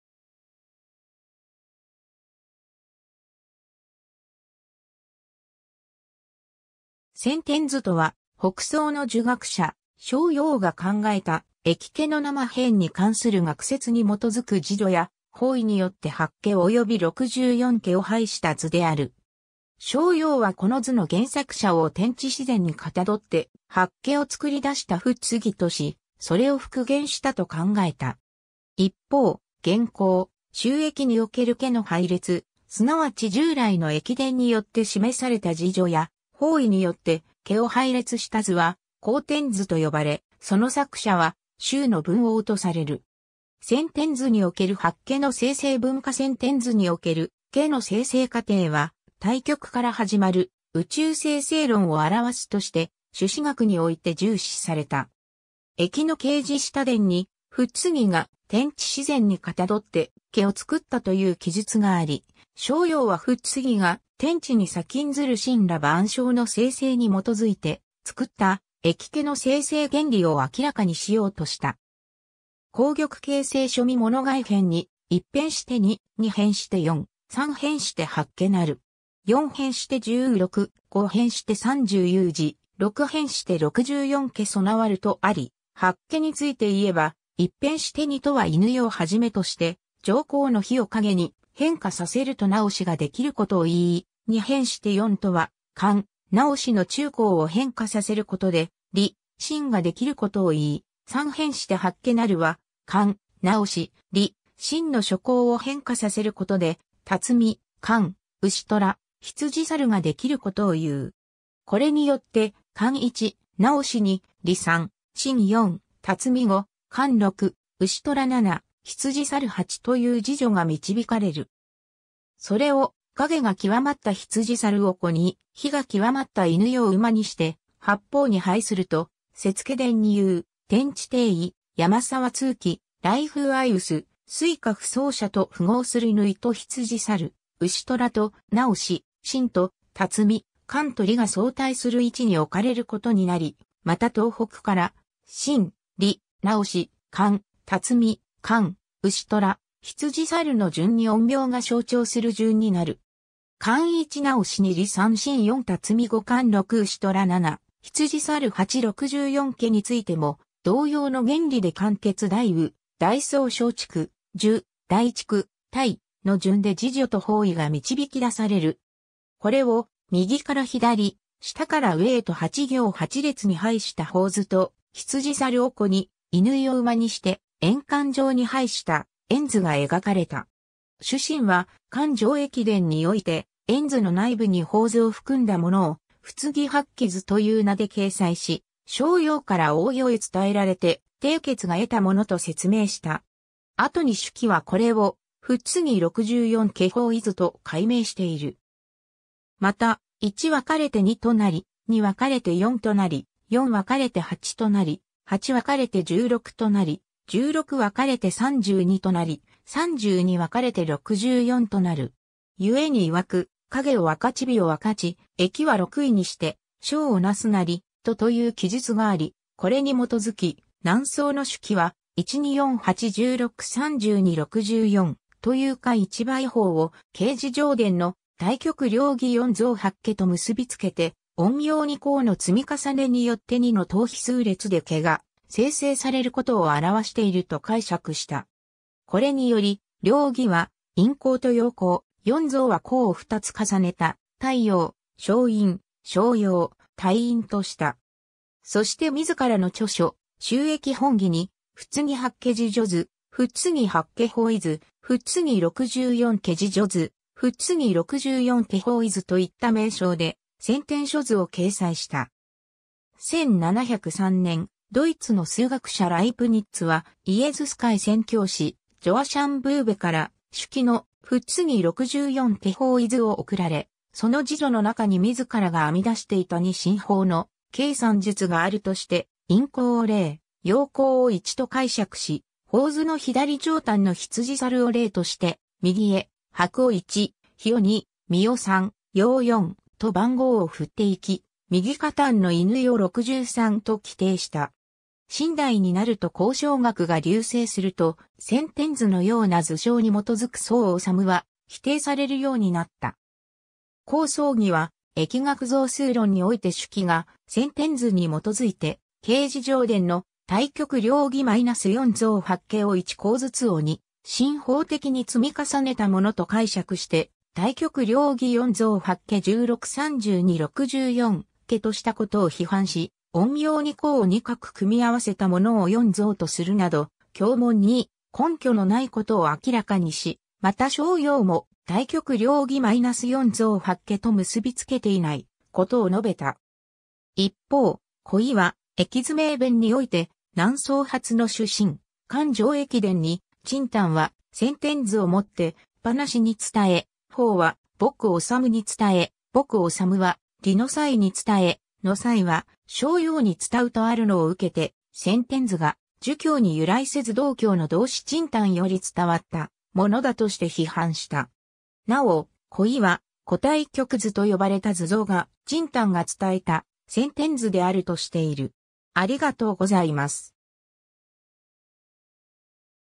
先天図
先天図（せんてんず）とは、北宋の儒学者邵雍が考えた易卦の生変に関する学説にもとづく次序や方位によって八卦および六十四卦を配した図である。
邵雍はこの図の原作者を天地自然に象って八卦を創り出した伏羲とし、それを復元したと考えた。
一方、現行『周易』における卦の配列、すなわち従来の易伝によって示された次序や方位によって卦を配列した図は後天図（こうてんず）と呼ばれ、その作者は周の文王とされる。
 先天図における八卦の生成分化 先天図における卦の生成過程は太極から始まる宇宙生成論を表すとして朱子学において重視された。
 易の繋辞下伝に伏羲が天地自然に象って卦を作ったという記述があり、邵雍は伏羲が「天地に先んずる」森羅万象の生成にもとづいて作った易卦の生成原理を明らかにしようとした。
『皇極経世書』観物外篇に「一変して二、二変して四、三変して八卦成る。四変して十有六、五変して三十有二、六変して六十四卦備わる」とあり、八卦について言えば、「一変して二」とは乾を始めとして上爻の陽を陰に変化させると兌ができることをいい、「二変して四」とは乾・兌の中爻を変化させることで離・震ができることをいい、「三変して八卦成る」は乾・兌・離・震の初爻を変化させることで巽・坎・艮・坤ができることをいう。
これによって乾一・兌二・離三・震四・巽五・坎六・艮七・坤八という次序が導かれる。
それを陰が極まった坤を子（北、下）に、陽が極まった乾を午（南・上）にして八方に配すると、説卦伝にいう「天地定位、山沢通気、雷風相薄、水火不相射」と符合する乾と坤、艮と兌、震と巽、坎と離が相対する位置に置かれることになり、また東北（右下）から震・離・兌・乾・巽・坎・艮・坤の順に陰陽が消長する順になる。
 乾一 兌二 離三 震四 巽五 坎六 艮七 坤八 六十四卦についても同様の原理で乾→夬→大有・大壮→小畜・需・大畜・泰…の順で次序と方位が導き出される。
これを右から左、下から上へと8行8列に配した方図と坤を子（北、下）に、乾を午（南・上）にして円環状に配した円図が描かれた。
朱震は『漢上易伝』において円図の内部に方図を含んだものを「伏羲八卦図」（ここでいう八卦とは六十四卦を統言する語）という名で掲載し、邵雍から王豫へ伝えられて鄭夬が得たものと説明した。
後に朱熹はこれを「伏羲六十四卦方位図」と改名している。
 また「一分かれて二と為り，二分かれて四と為り、四分かれて八と為り、八分かれて十六となり、十六分かれて三十二と為り、三十二分かれて六十四と為る。故に曰く、陰を分かち陽を分かち、易は六位にして章を成すなり、と」という記述があり、これにもとづき南宋の朱熹は「1→2→4→8→16→32→64」という加一倍法を繋辞上伝の「太極→両儀→四象→八卦」と結びつけて、陰陽2爻の積み重ねによって2の等比数列で卦が生成されることを表していると解釈した。
これにより両儀は陰爻と陽爻、四象は爻を二つ重ねた太陽・少陰・少陽・太陰とした。
そして自らの著書『周易本義』に「伏羲八卦次序図」「伏羲八卦方位図」「伏羲六十四卦次序図」「伏羲六十四卦方位図」といった名称で先天諸図を掲載した。
1703年、ドイツの数学者ライプニッツはイエズス会宣教師ジョアシャン・ブーヴェから朱熹の「伏羲六十四卦方位図」を送られ、その次序の中に自らが編み出していた2進法の計算術があるとして、陰爻 ¦ を0、陽爻 | を1と解釈し、方図の左上端の坤 ¦¦¦¦¦¦ (000000)を0として右へ剥 ¦¦¦¦¦| (000001)を1、比 ¦¦¦¦|¦ (000010)を2、観 ¦¦¦¦|| (000011)を3、豫 ¦¦¦|¦¦ (000100)を4…と番号を振っていき、右下端の乾 (111111)を63と規定した。
 清代になると考証学が隆盛すると、先天図のような図象にもとづく宋易は否定されるようになった。
黄宗羲は『易学象数論』において朱熹が先天図にもとづいて繋辞上伝の「太極-両儀-四象-八卦」を1爻ずつを2進法的に積み重ねたものと解釈して「太極(1)→両儀(2)→四象(4)→八卦(8)→16→32→六十四卦(64)」としたことを批判し、陰陽2爻を2画組み合わせたものを四象とするなど経文に根拠のないことを明らかにし、また邵雍も太極-両儀-四象-八卦と結びつけていないことを述べた。
一方、胡渭は『易図明辨』において南宋初の朱震『漢上易伝』に「陳摶は「『先天図』を以て种放に伝え、放は穆脩に伝え、穆脩は李之才に伝え、之才は邵雍に伝う」とあるのを受けて先天図が儒教に由来...